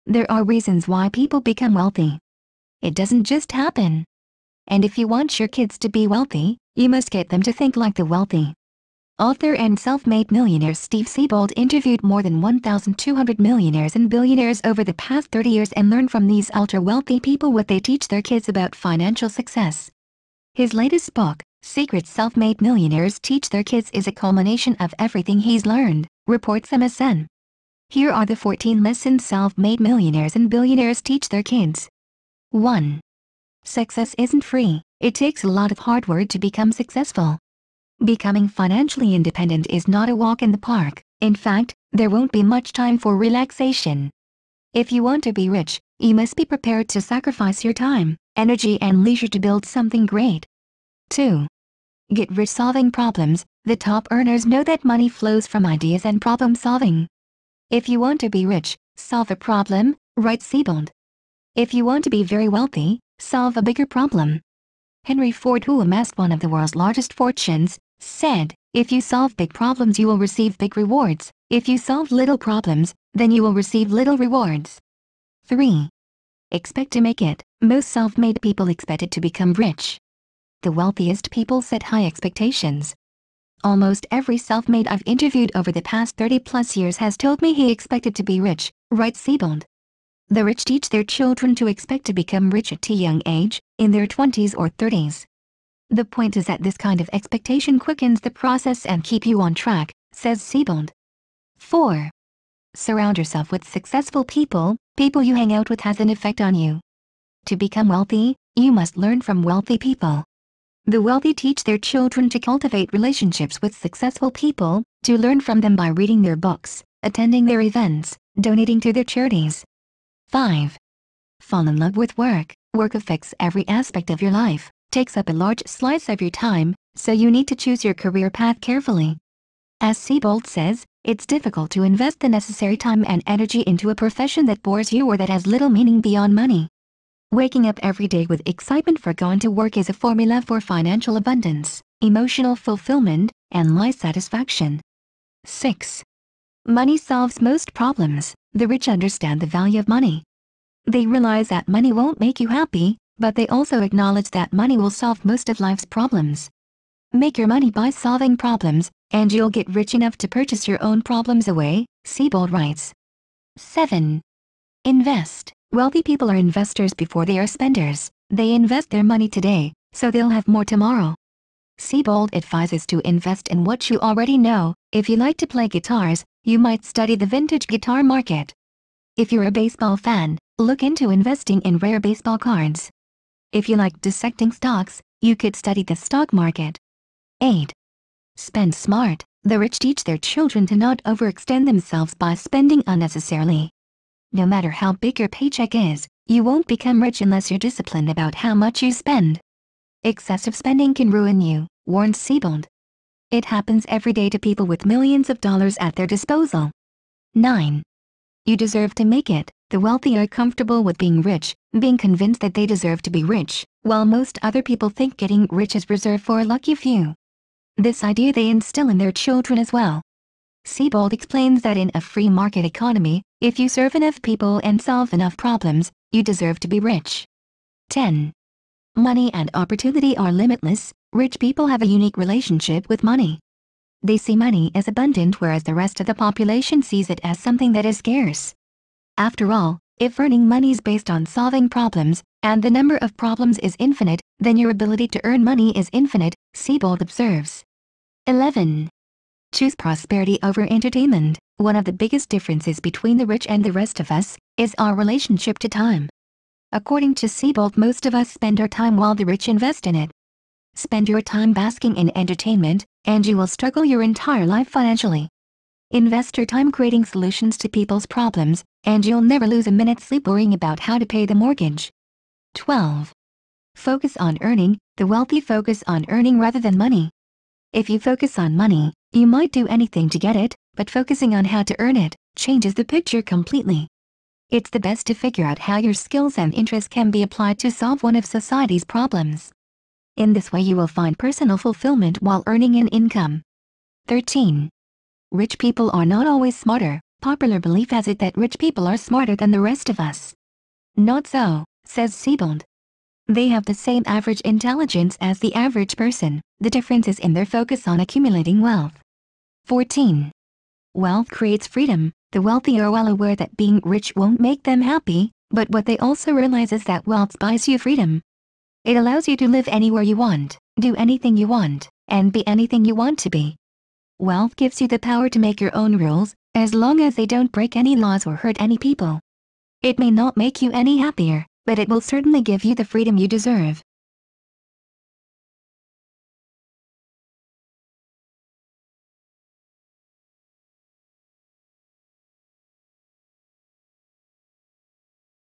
14 principles millionaires and billionaires teach their kids about money
There are reasons why people become wealthy. It doesn’t just happen. And if you want your kids to be wealthy, you must get them to think like the wealthy.

Author and self-made millionaire Steve Siebold interviewed more than 1,200 millionaires and billionaires over the past 30 years and learned from these ultra-wealthy people what they teach their kids about financial success.

His latest book, Secrets Self-Made Millionaires Teach Their Kids is a culmination of everything he’s learned, reports MSN.